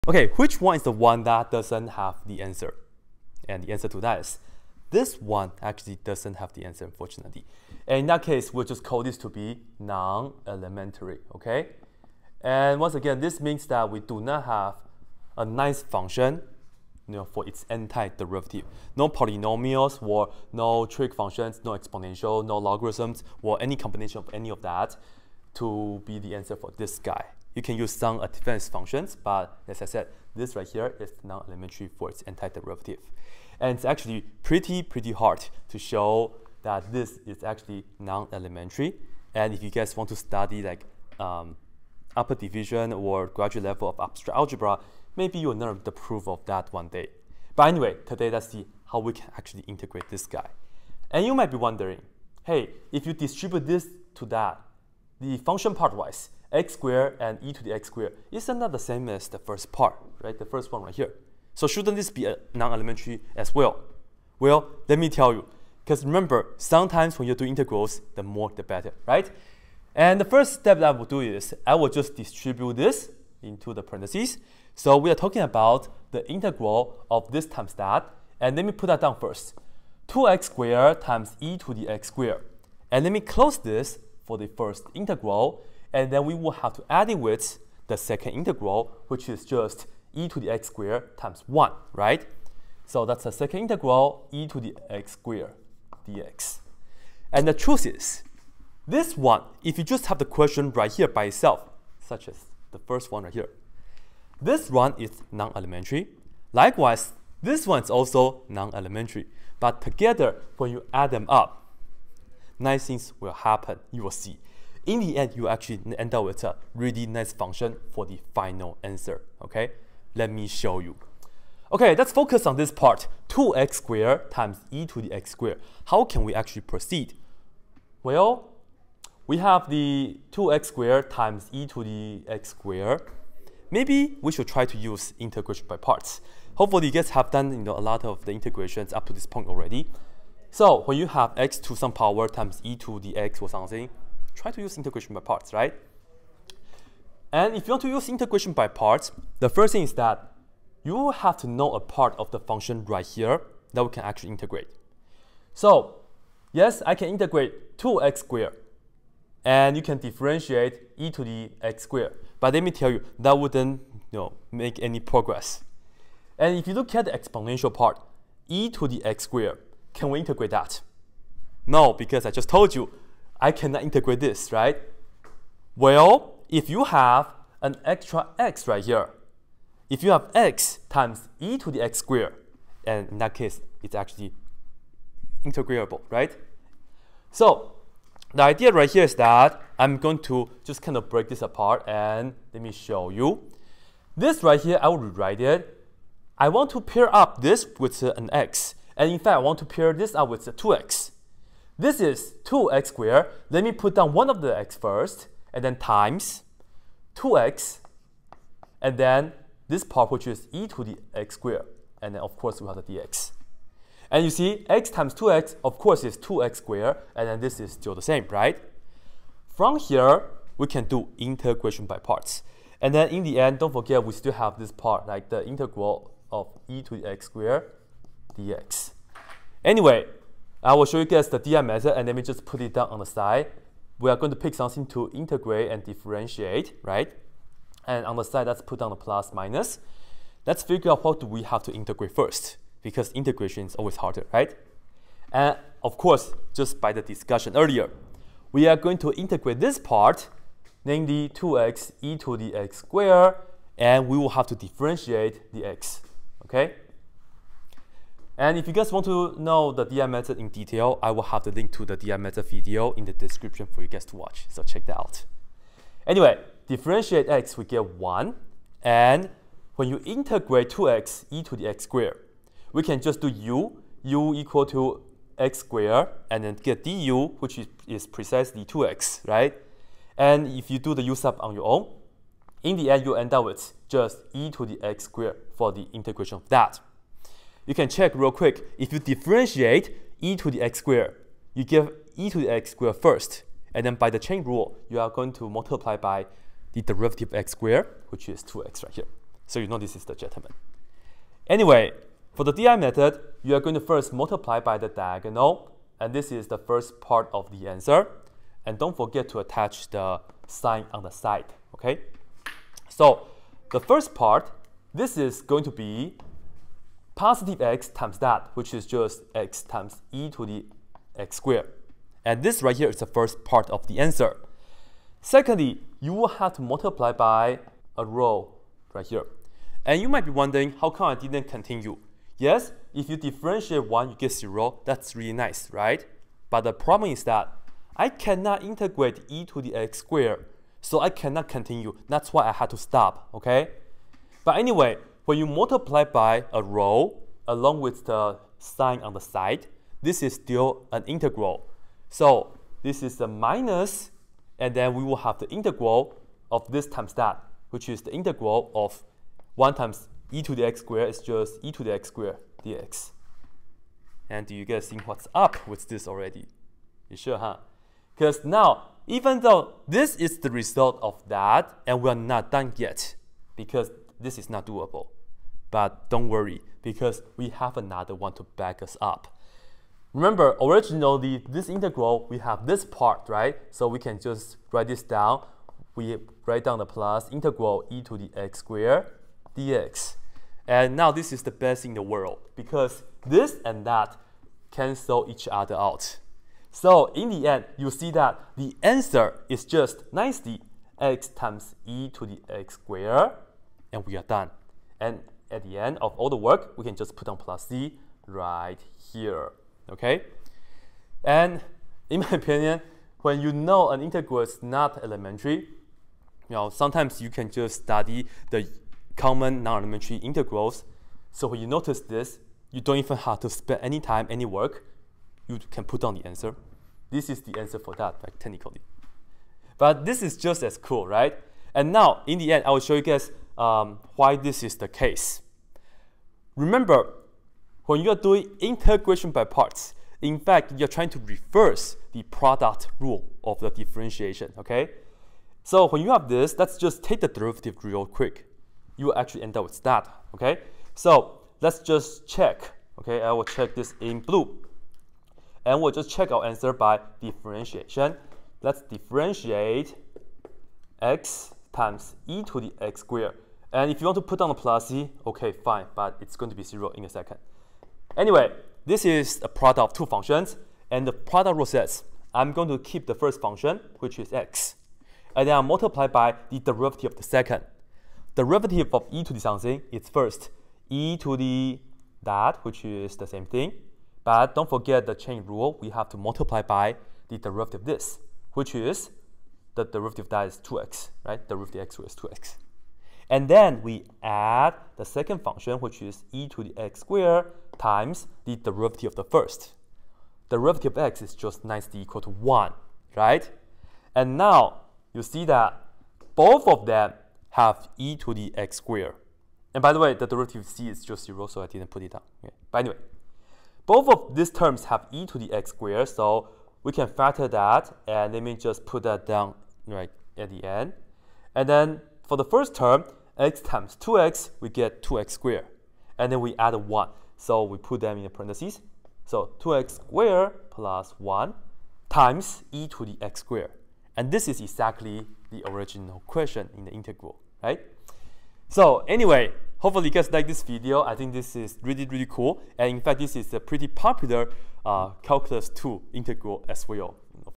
Impossible Calculus 2 integral?
integral of (2x^2+1)e^(x^2),

blackpenredpen,
math for fun,